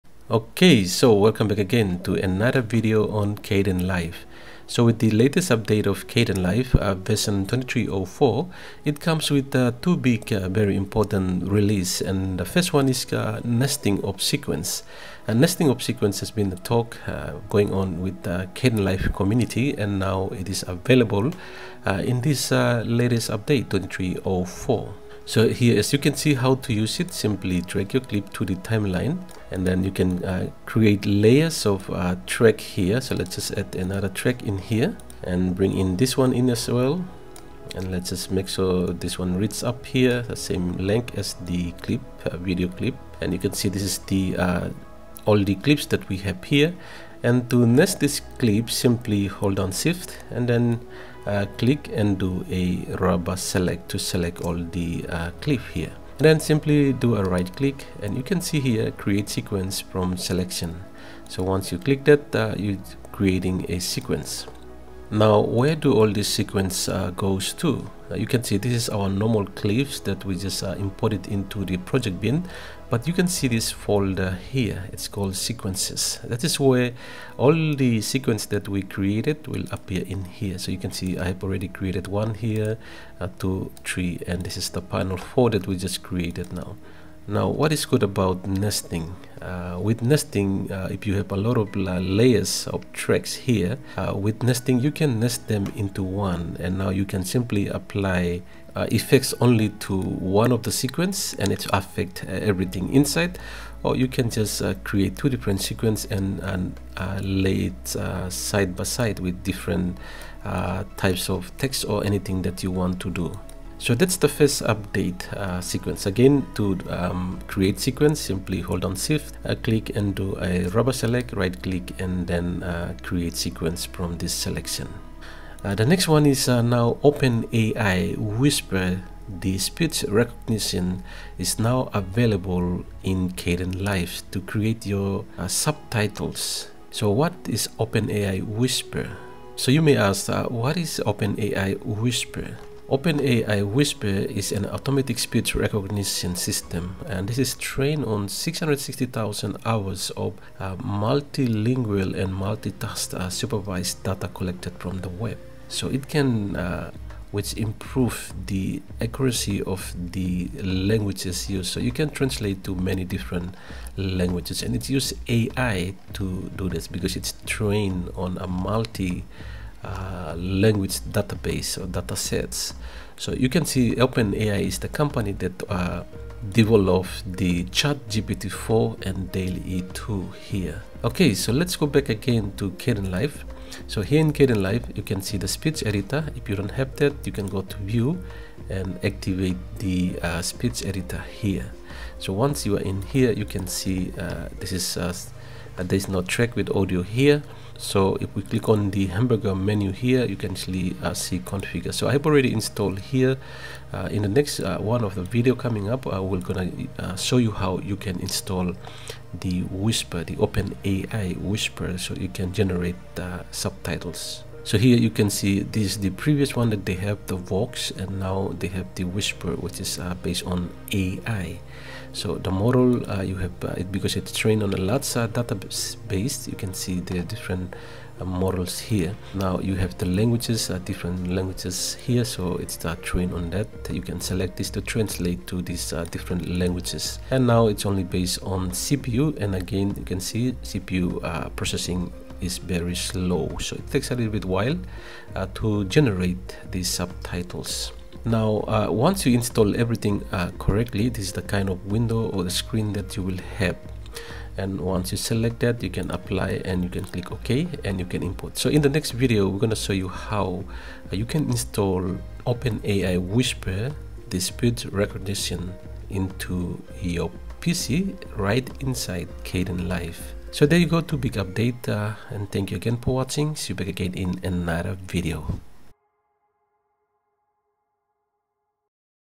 OpenAI Whisper in Kdenlive Speech Editor
With the latest update of kdenlive 23:04 comes with two big updates. The Nesting Sequence and OpenAI Whisper, the most accurate speech recognition.  OpenAI  is the company behind ChatGPT.  What it means for Kdenlive user? Most accurate speech translations. In the next video we show you how you can install it.